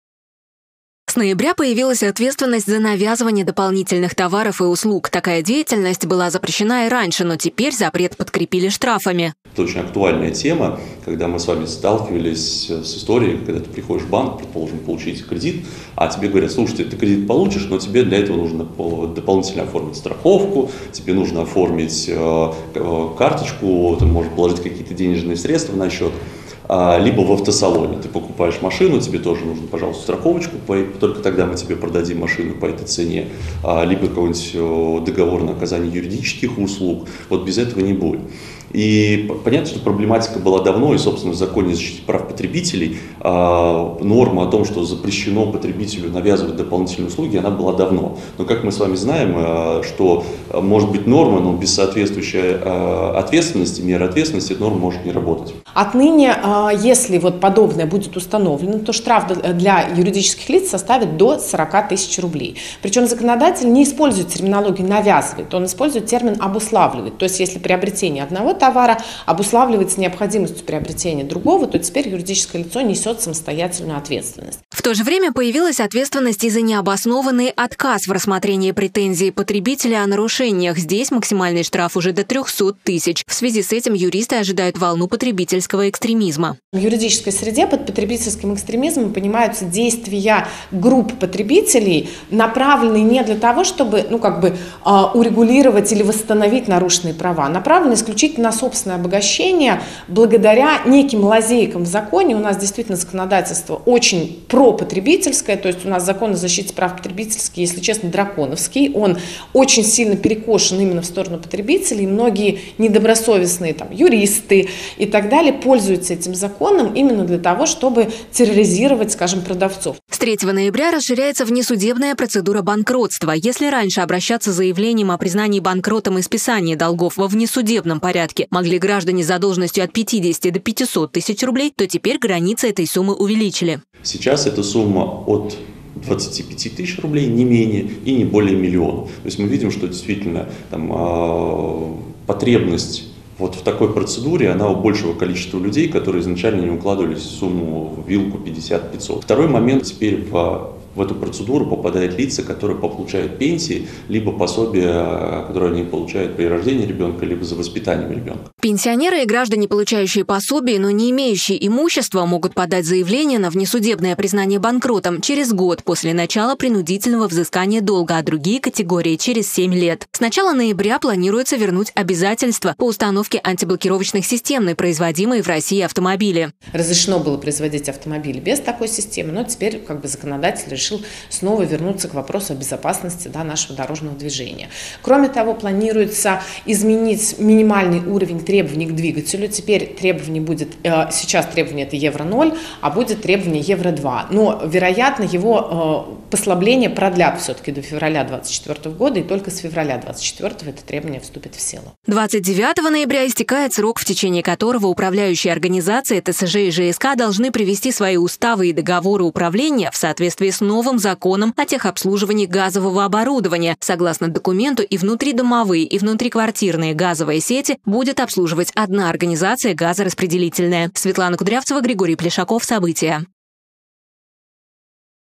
В России вступили в силу новые законодательные изменения
Дополнительная защита граждан от недобросовестных продавцов и расширение внесудебной процедуры банкротства. В России вступили в силу новые законодательные изменения. Их обсудила с экспертами Светлана Кудрявцева.